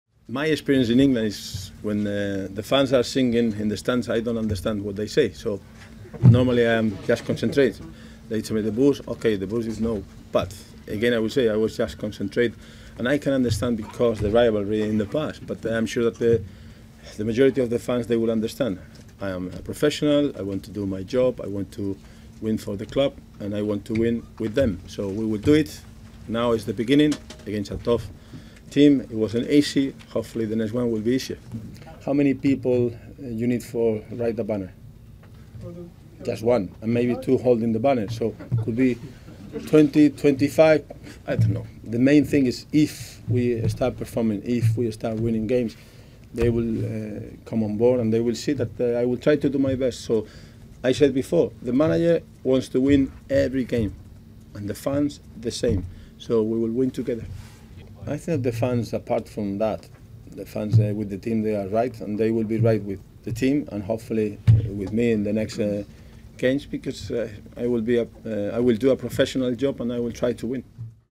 Chelsea 0-0 Man City: Benitez expects fans to come around
New Chelsea boss Rafa Benitez says he's confident he can win the club's fans round when he starts winning games.